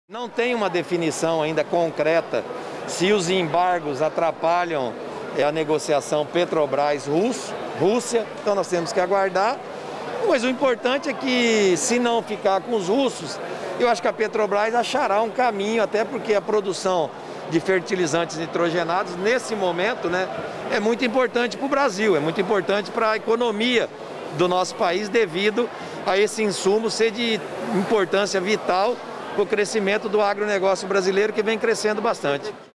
Governador deixa claro que conclusão da UFN3, comprada por russos, pode ser frustrada
Durante a entrega dos equipamentos para a segurança pública, o Governador deixou claro que não existe mais a garantia de que os russos vão mesmo concluir a fábrica de fertilizantes de Três Lagoas.